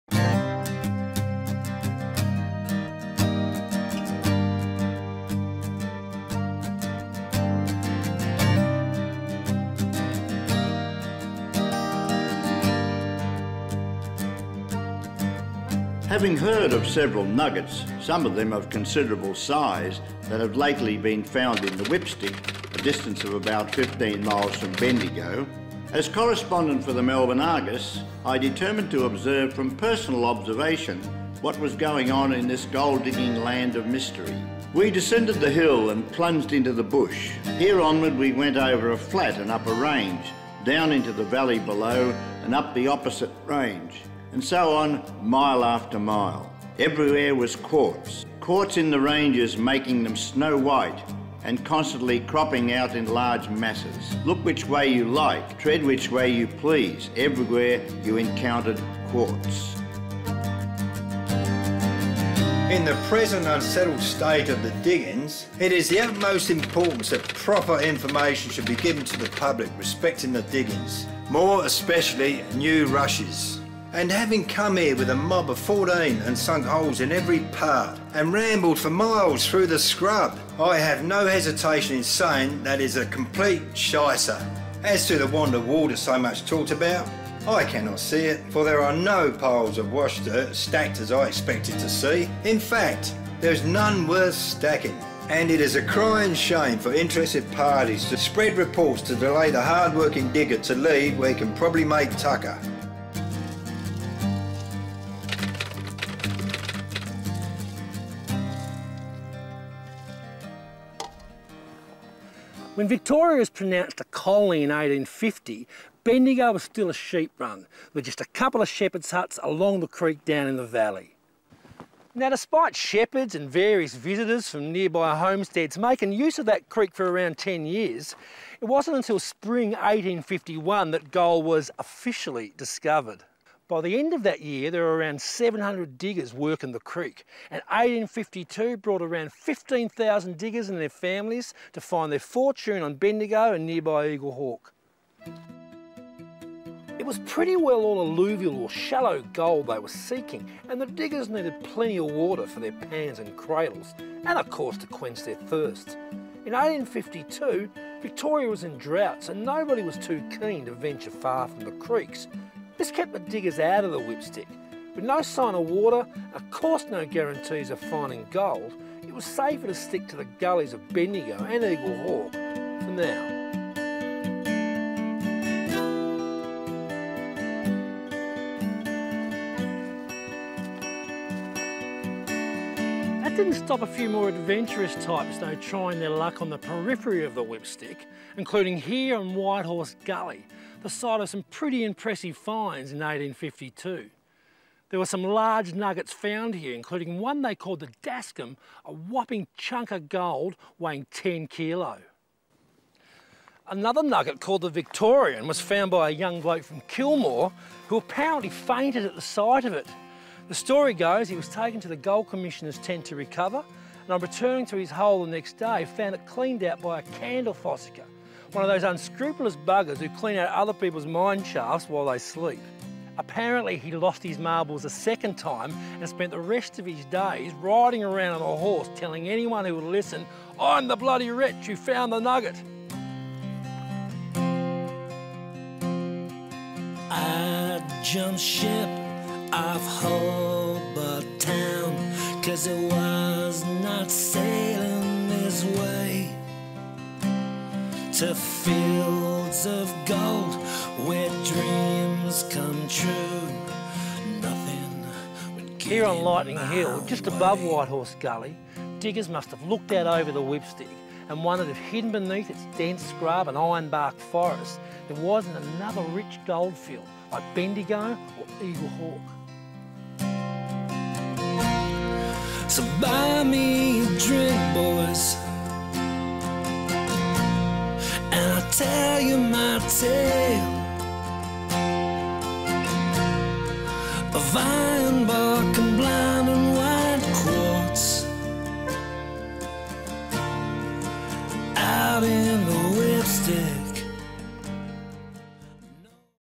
Gold Rushes of the Whipstick - Part One
Today we're bringing you the fascinating history of the gold rushes in the Whipstick!

Join Jamie as we journey back in time to explore the stories and legends behind one of the most exciting periods in Australia's history.

With stories from the past and stunning footage of the landscape, you'll get a firsthand look at the challenges and rewards of seeking fortune in this rugged terrain.

Whether you're a history buff or a gold prospecting enthusiast, this video is sure to captivate and inspire. So, sit back and join us as we take you on an unforgettable journey through the gold rushes of the Whipstick!